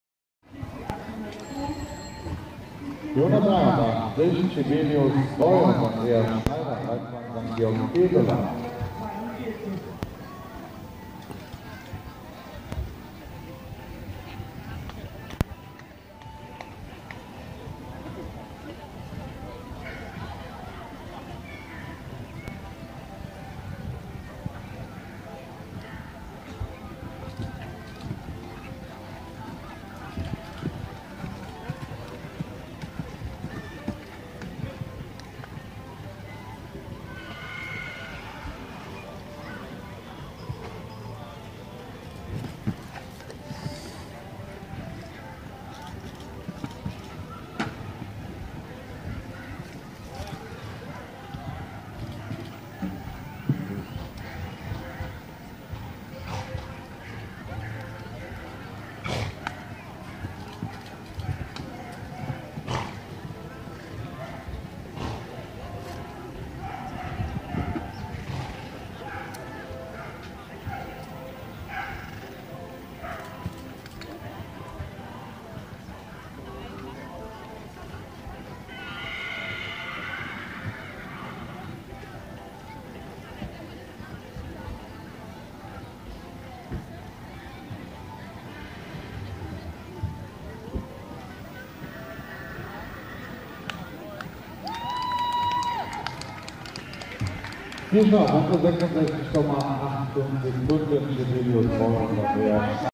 M*-Springen mit Cebi in Sevelen
Es war echt anspruchsvoll gebaut und bin sehr zufrieden trotz Klotz. Etwas schneller und ich wäre sogar noch platziert gewesen.